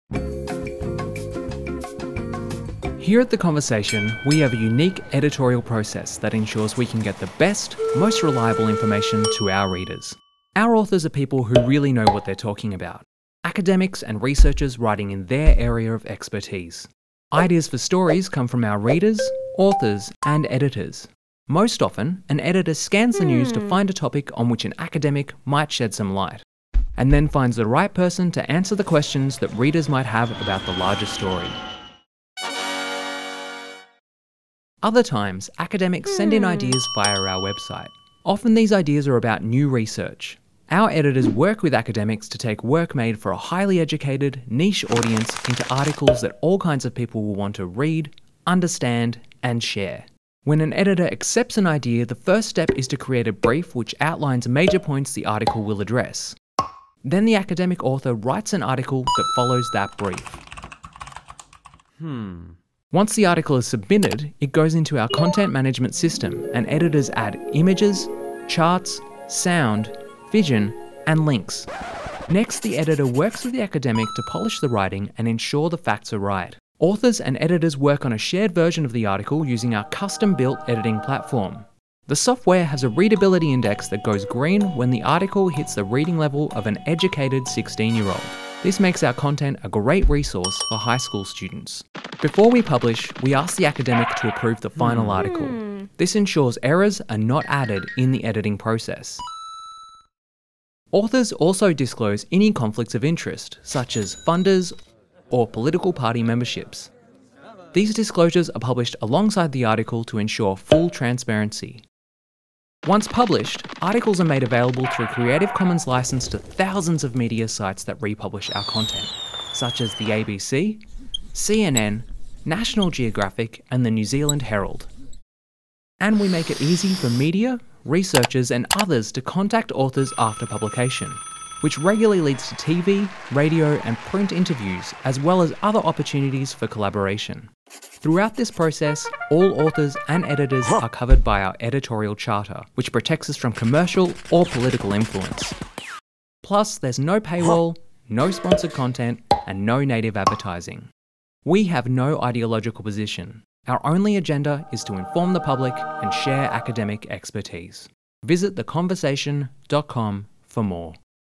How we work at The Conversation
The Conversation isn't like most news organisations and is unique in how its pieces are commissioned, edited and published.

We exclusively publish academics, and ensure that everything on our site is evidence-based and pulled from the latest research and data.

From the minute an academic pitches their idea, to the moment we press publish on an article, the process can be a little tricky to understand.

And that's why we produced this short video to take you through how it all works.

Edited and animated by Wes Mountain.
Script by Misha Ketchell, Debbie Dickinson, Sunanda Creagh and Fron Jackson-Webb.